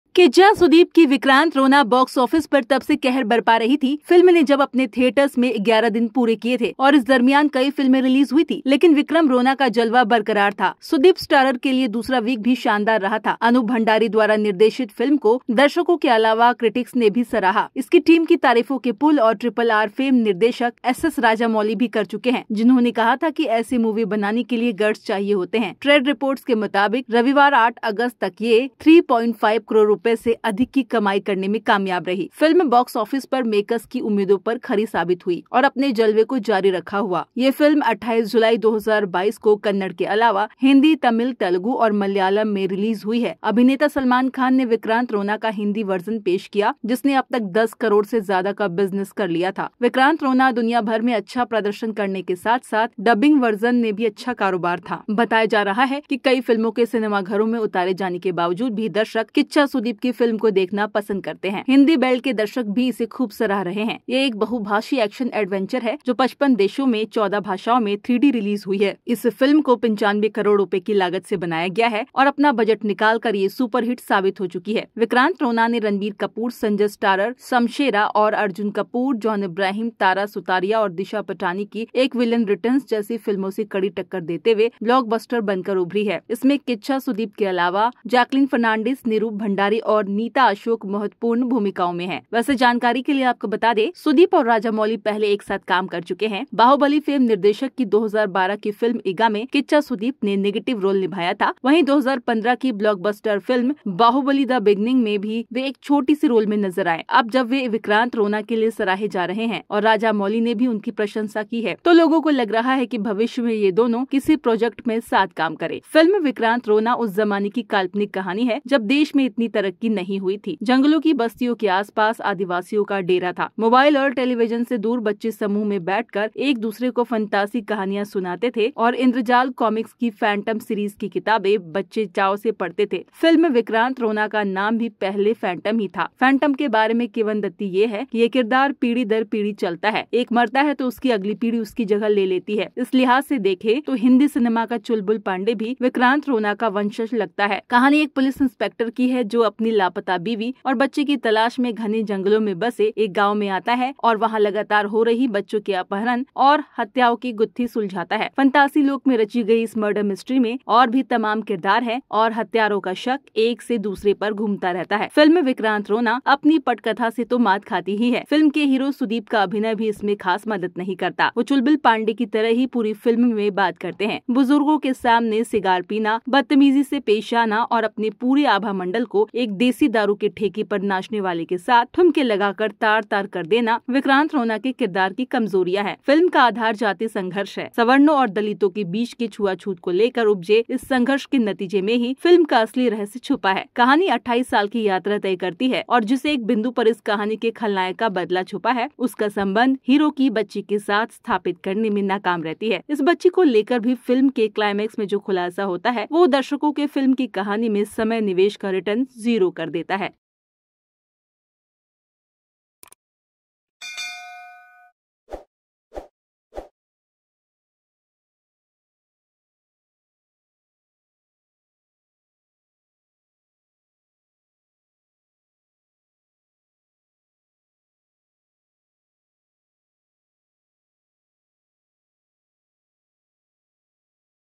Kichcha Sudeep की 'Vikrant Rona' का Box office पर जलवा बरकररार..
Welcome to @BRCTV

Kichcha Sudeep की 'Vikrant Rona' का Box office पर जलवा बरकररार, कमाए इतने करोड़ रुपए; फिर पिछड़ा बॉलीवुड